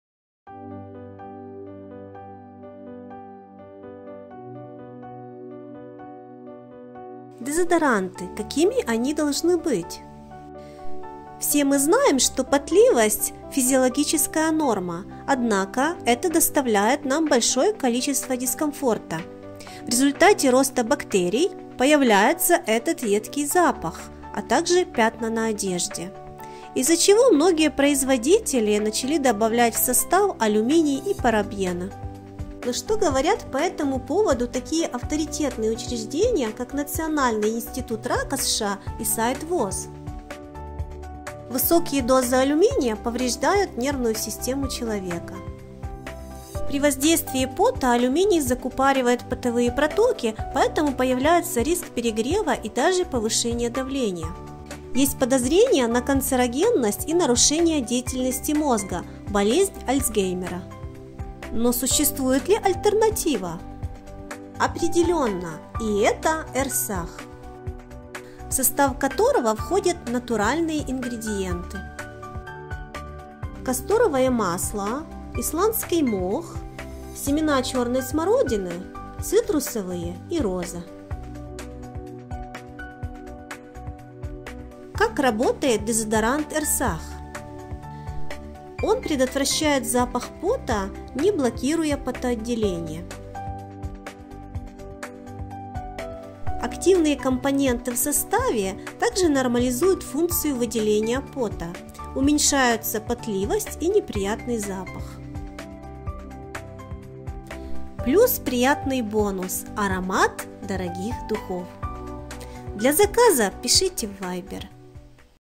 ДЕЗОДОРАНТЫ. КАКИМИ ОНИ ДОЛЖНЫ БЫТЬ?
ДЕЗОДОРАНТЫ ЭРСАГ ЕСТЬ ДВУХ ВАРИАНТОВ:

- ШАРИКОВЫЕ

✅Код 213 Женский шариковый дезодорант (имеет приятный, лёгкий цветочный аромат)
✅Код 214 Мужской шариковый дезодорант (приятный, нежный мужской аромат)
✅Код 156 Унисекс (приятный европейский аромат)

- ДЕО-СПРЕЙ

🍃Код 301 Свежесть "Fresh" (обладает приятным ненавязчивым ароматом свежести)
🍃Код 302 Экзотик (экзотический, приятный аромат с нотками цитруса, гарантирует чувство свежести на целый день)